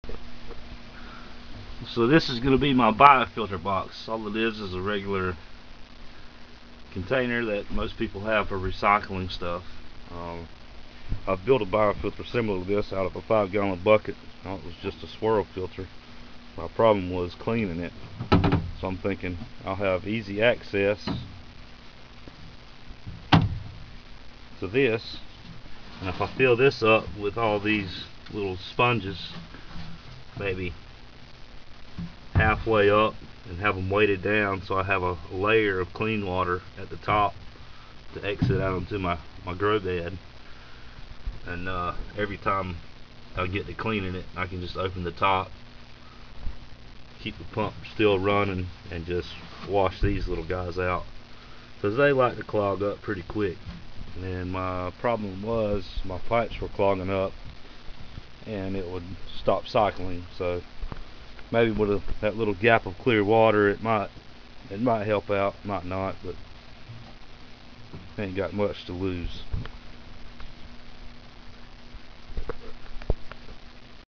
bio filter idea
trying to figure out how to add a bio filter (swirl type) to the new system,  just a rough idea,